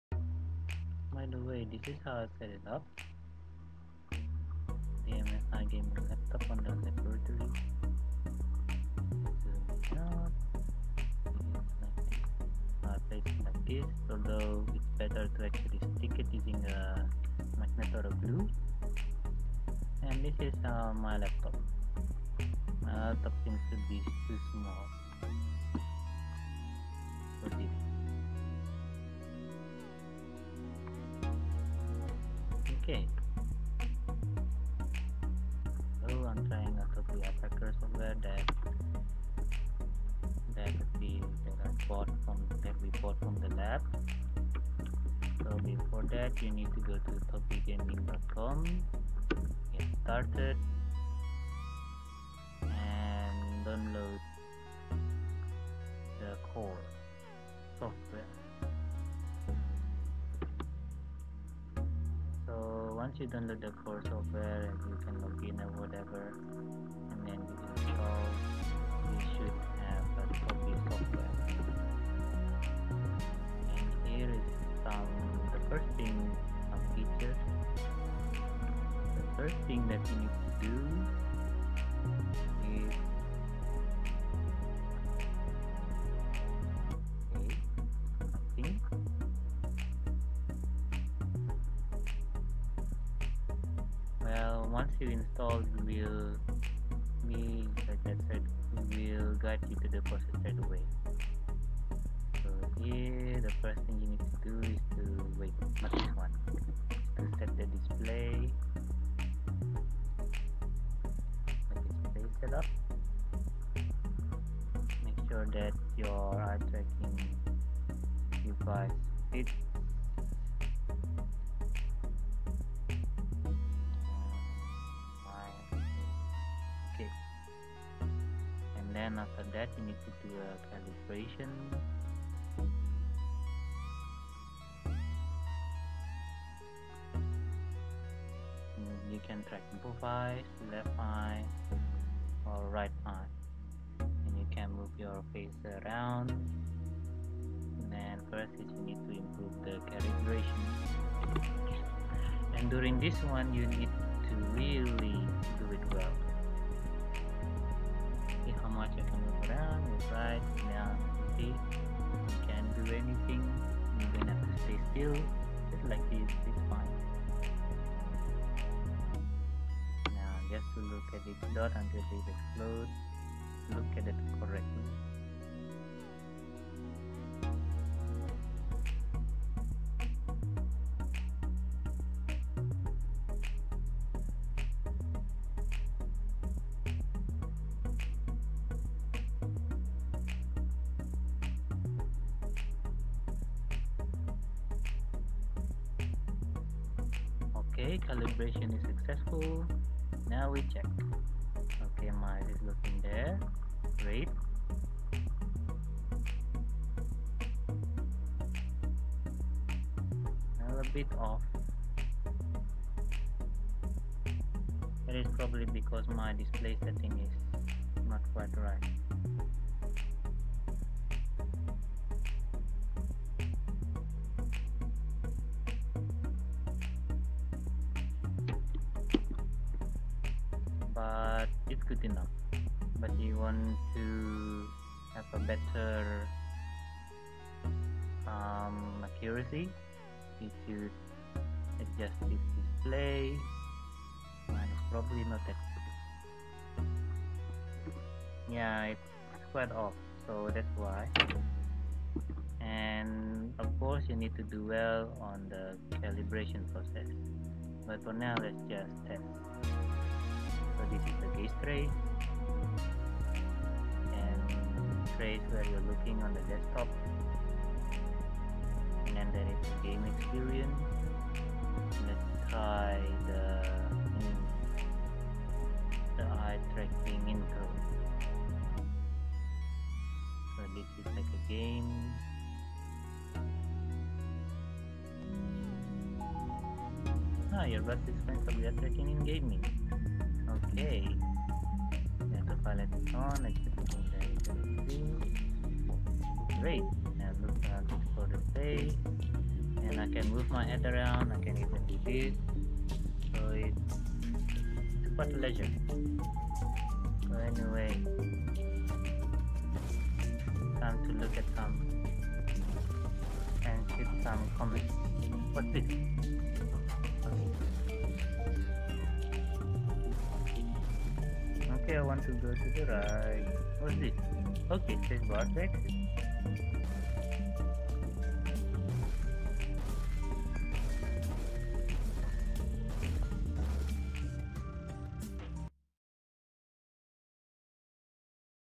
Trying Tobii Eye Tracker 4c on Windows 10
We just got Tobii Eye Tracker 4c and tried this on Windows 10. and managed to visualize the eye tracking on the desktop.

00:00 device setup
00:40 download software
01:10 open software
01:35 calibration
04:15 activate eye tracking
04:35 sample game